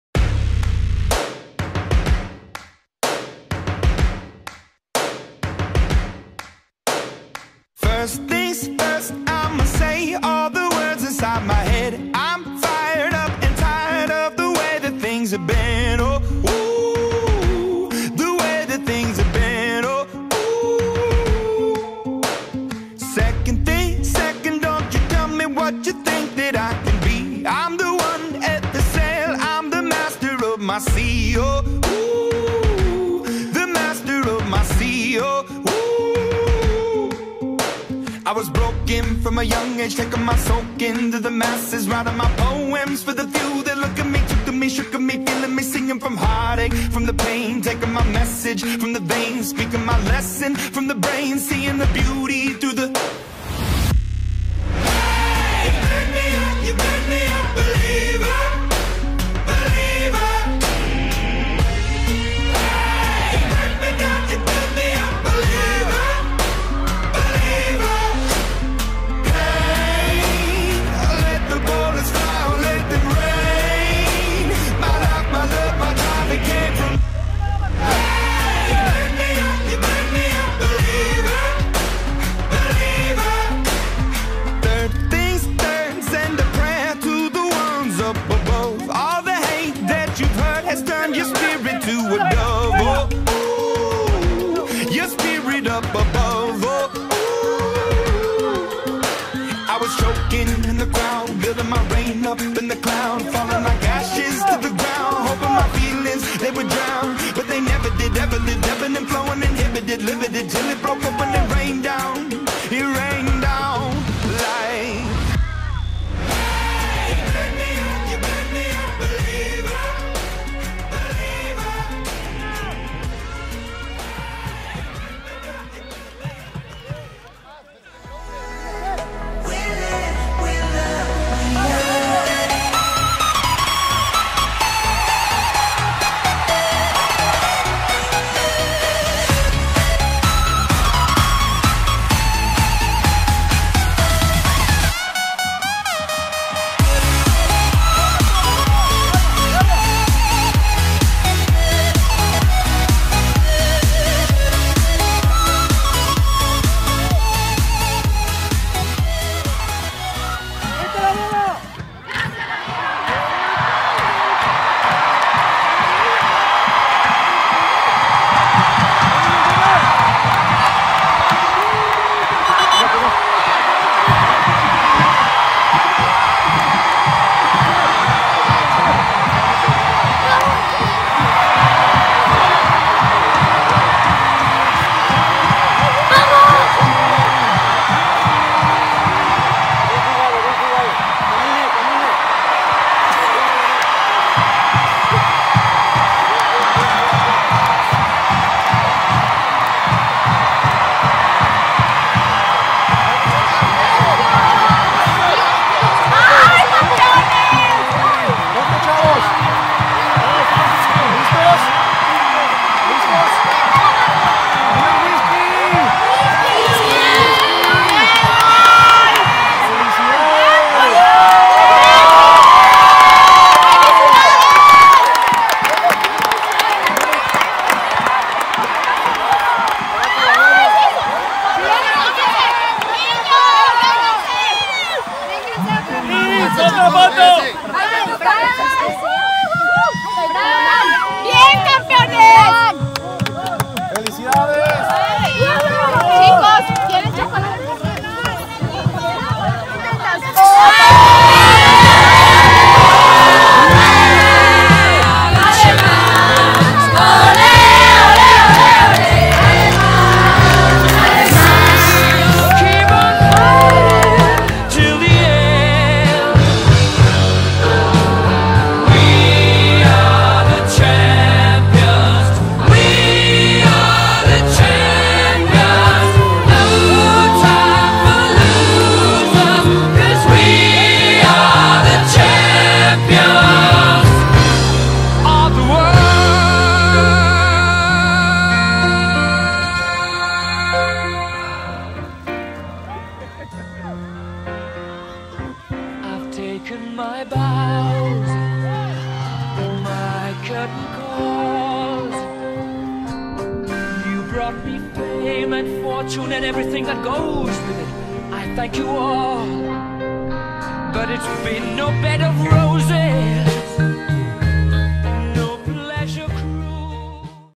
Final Futbol 2019 - Liga Córdica (Colegio Aleman vs Cumbres San Javier)
Campeonato de Futbol - Liga Córdica 2019
Colegio Aleman Guadalajara vs. Colegio Cumbres San Javier